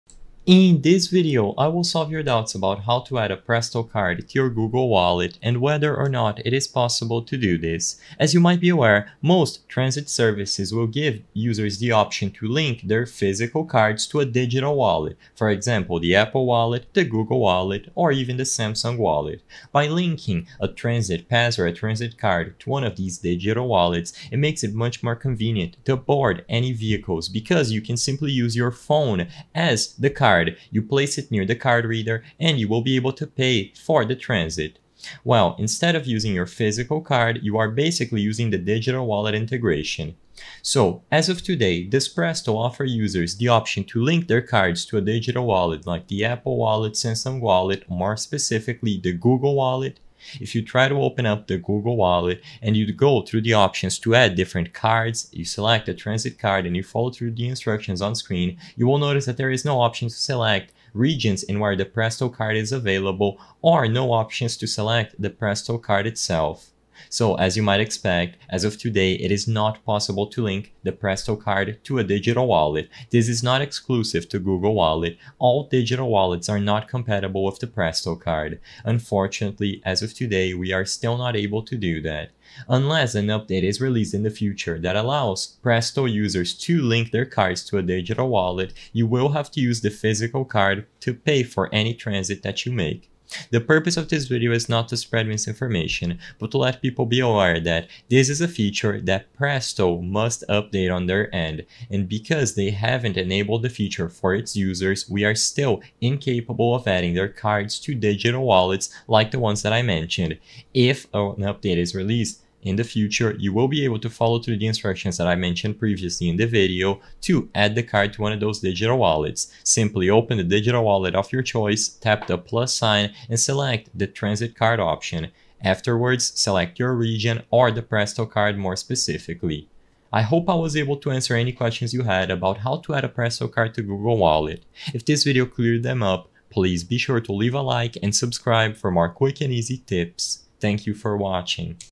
How To Add Presto Card to Google Wallet? (2025)
In this video I will solve your doubts about how to add presto card to google wallet, and whether or not it is possible to do this.

I hope I have helped you with the video!